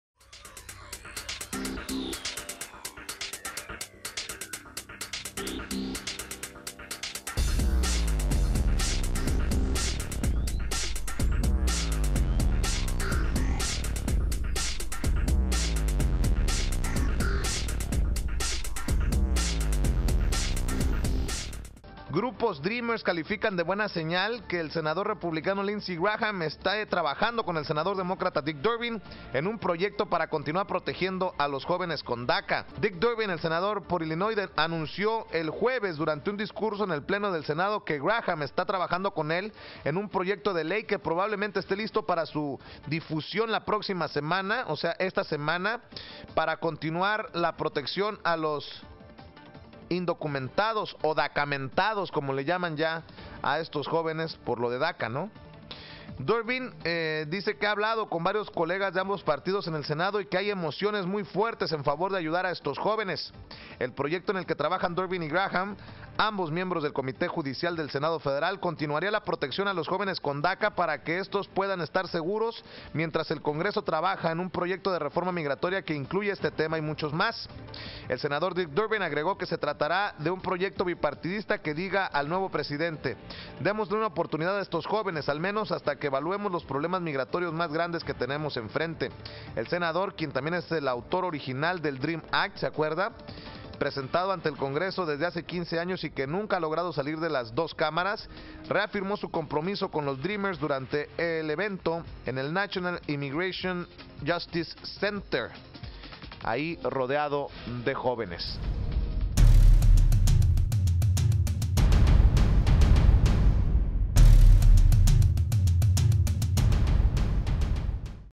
Senador Durbin prepara medida para proteger a “Dreamers"
El senador demócrata por Illinois, Dick Durbin, afirmó este lunes que presentará una medida para proteger a los “Dreamers” amparados bajo el “DACA” de 2012.